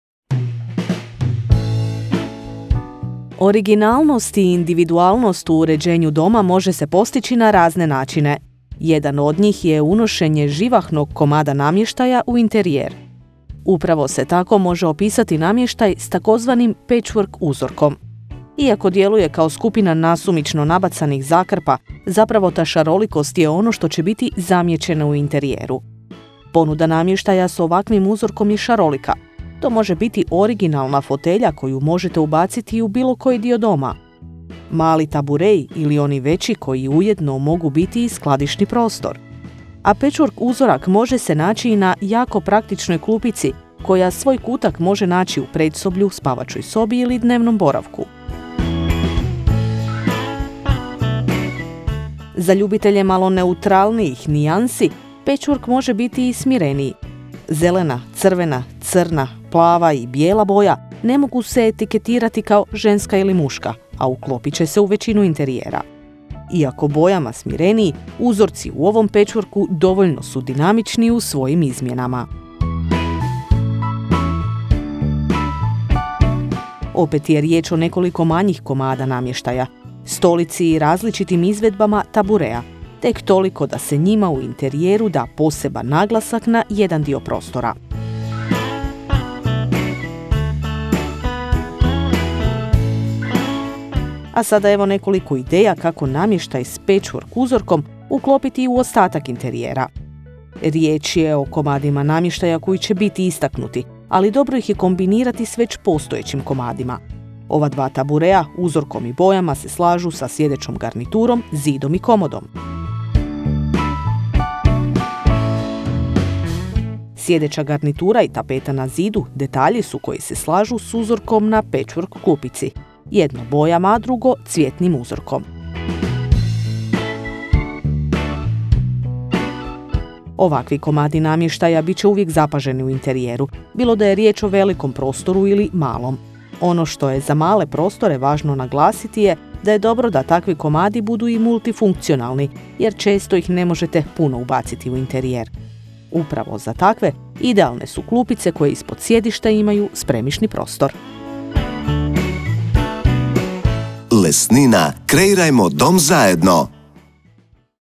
Ušarenite dom ovim komadima namještaja
Individualnost je već neko vrijeme u modi u uređenju interijera. A nju ćete najbolje pokazati upečatljivim komadom namještaja. To može biti fotelja i tek mala klupa ili tabure. Ovi komadi namještaja zasigurno neće proći neprimijećeni.